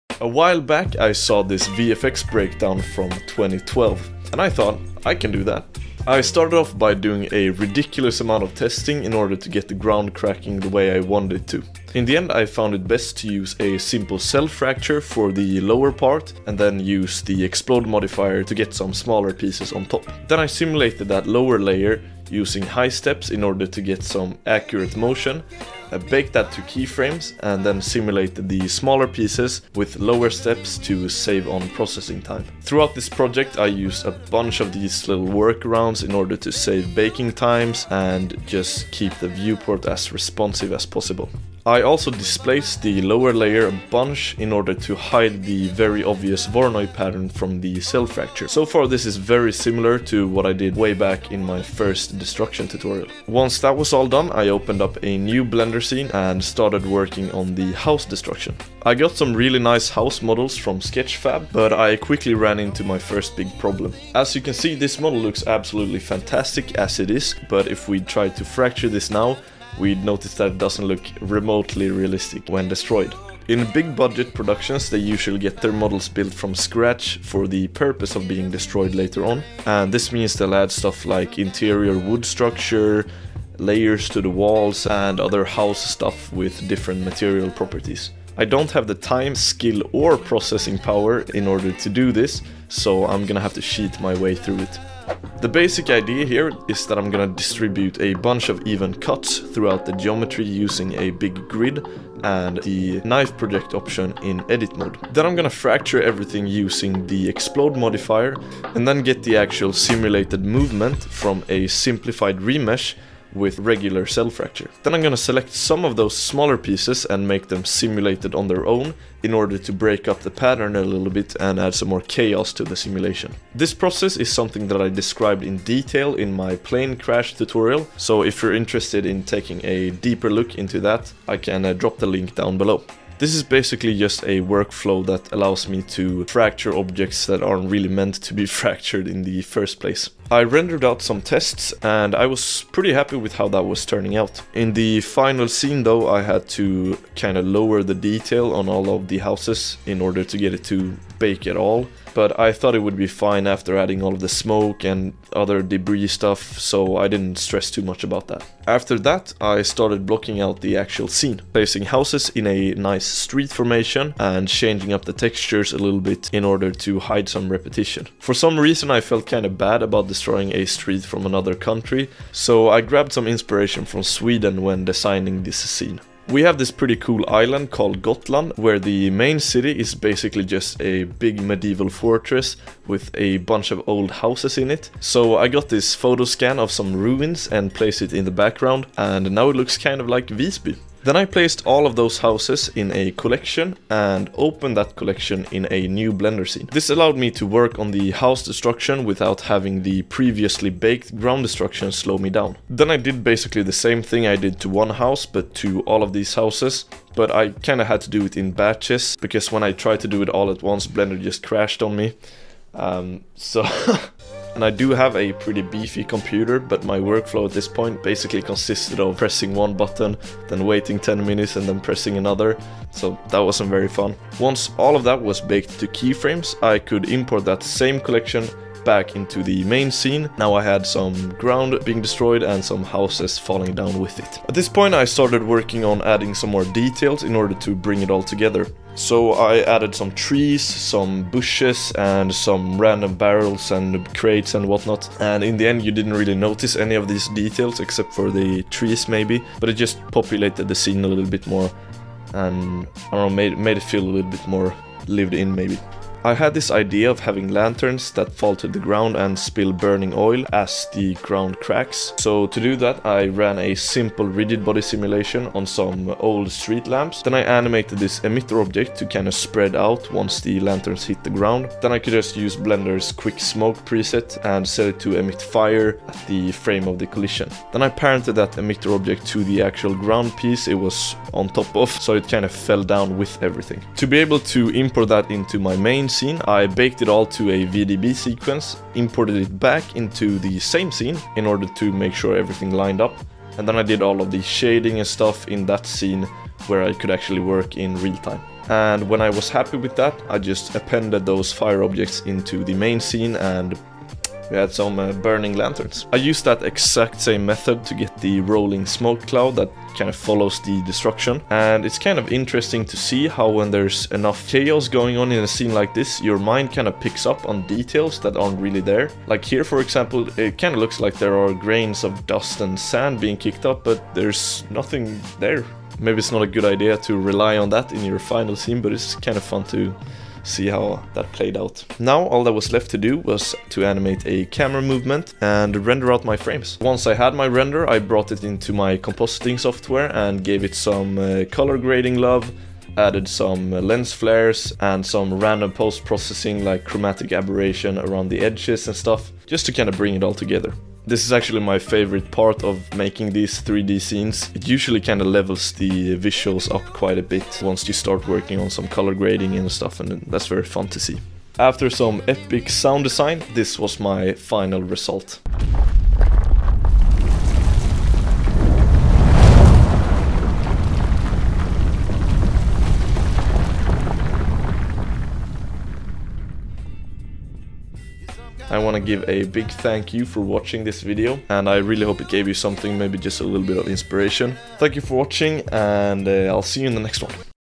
advanced destruction effects in blender
timestamps:
00:00 - Ground destruction
00:53 - Testing house destruction
02:40 - Blocking out scene
03:11 - House destruction
04:16 - Burning lanterns
05:30 - Compositing
06:00 - Final scene

Big thanks to Mr. The Rich for providing the house model in the thumbnail!